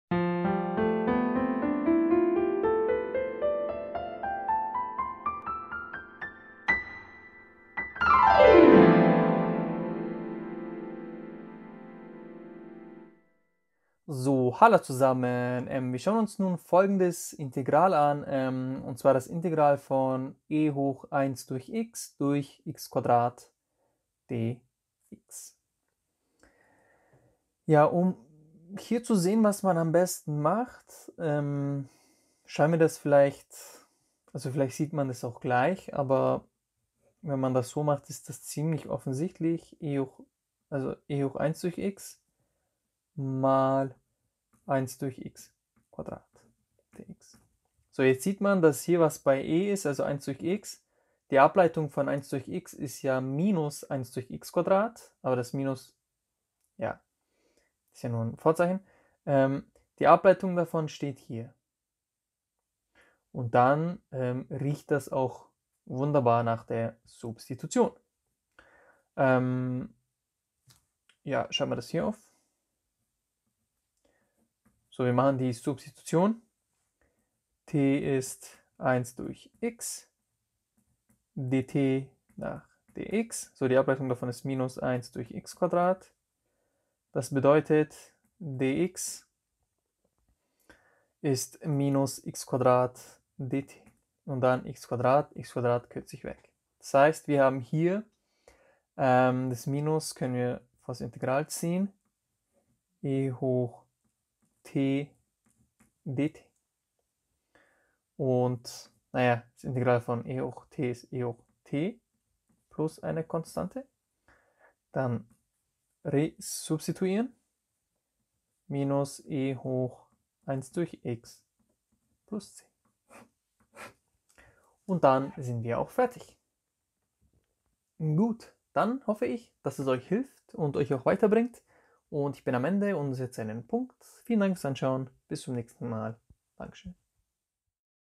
Integral e^(1/x)/x^2 | Substitution
Integral - lat. integralis "ganz, unversehrt, einschließend" - "Integrieren ist Kunst, Differenzieren ist Technik" - in diesem Sinne lösen wir das Integral e^(1/x)/x^2